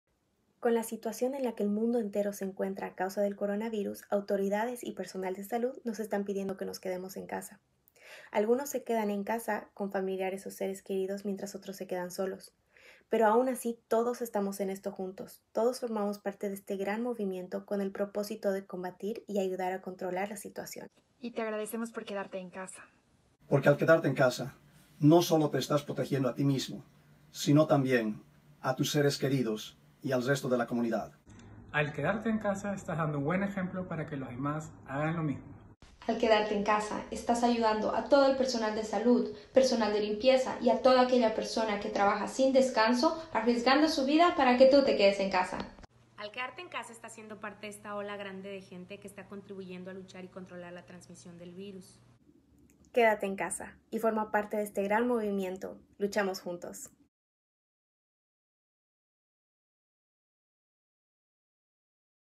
QUÉDATE EN CASA!
Al quedarte en casa:
- No solo nos estamos protegiendo a nosotros mismos, sino también a nuestros seres querido y al resto de la comunidad.
- Estamos dando el ejemplo para que otros también se queden en casa!
- Estamos ayudando al personal de salud, personal de limpieza y a todas aquellas personas que están trabajando sin descanso poniendo sus vidas en riesgo para que tu te quedes en casa a salvo.
- Estás formando parte de este gran movimiento que tiene como propósito ayudar a controlar el contagio del virus covıd19.
*Agradecimiento especial a todos mis colegas que contribuyeron para hacer este vídeo! @marielkach @marielita17 @joseantonio_mansen @dianapaulettesigala .